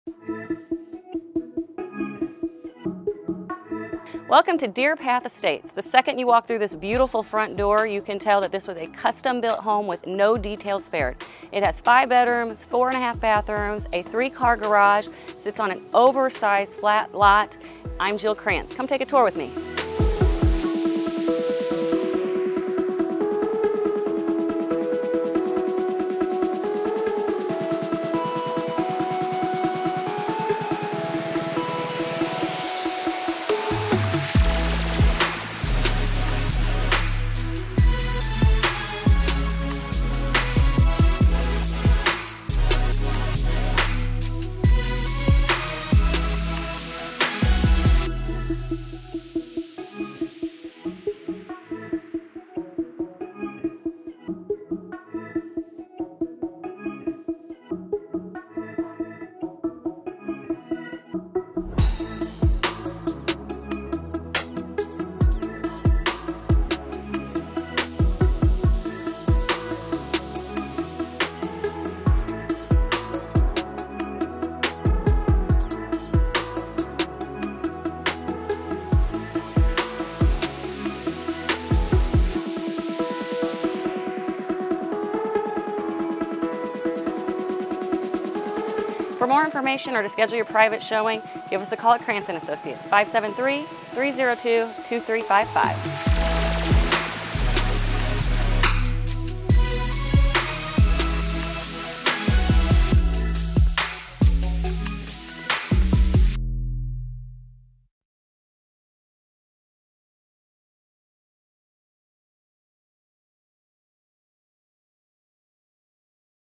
124 Antler Point, Linn Creek is the DREAM home!
124 Antler Point, Linn Creek

5 Bed | 4 Bath | 3165 SQFT

Welcome to your dream home! This 3,165 sq ft custom-built residence offers 5 bedrooms and 4.5 bathrooms on a beautifully landscaped, flat lot. The moment you enter, you'll notice the high-end finishes and attention to detail. The open floor plan is perfect for entertaining, featuring custom cabinetry, a gourmet kitchen with a gas stove, and a formal dining/hearth room. Elegant ceilings and engineered hardwood floors add warmth and sophistication. Enjoy the electric fireplace in the living area or relax on the spacious decks. The master suite boasts a walk-in closet, soaker tub, and dual vanities. An upstairs En Suite offers versatility as an office or playroom, while the lower level includes a 14x16 workshop with additional storage. This home is designed to meet all your needs, making it the perfect place to call HOME!